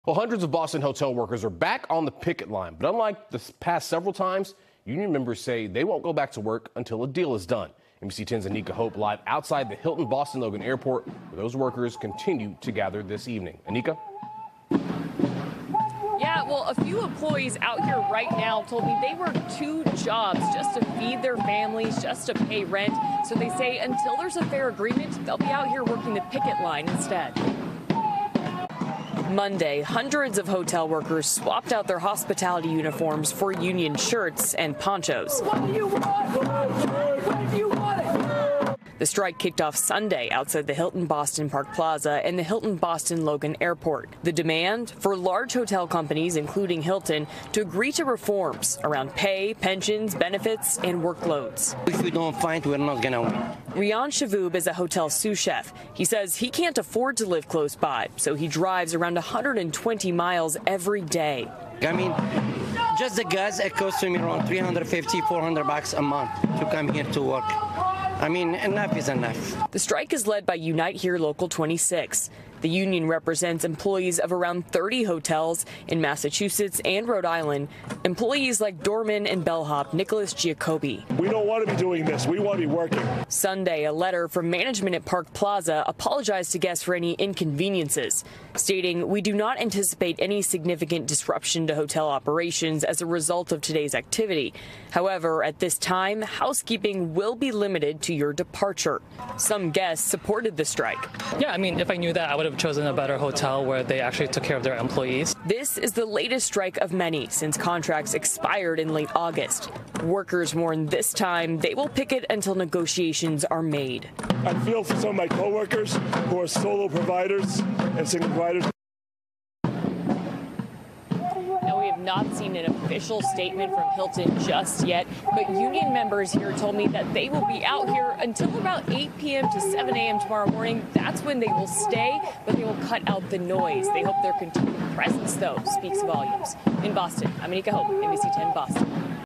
Boston hotel worker strike
Picketing continued Monday at two major Hilton hotels in Boston as the UNITE HERE Local 26 union continued its strike there in an effort to get higher pay, pension increases, better benefits and manageable workloads.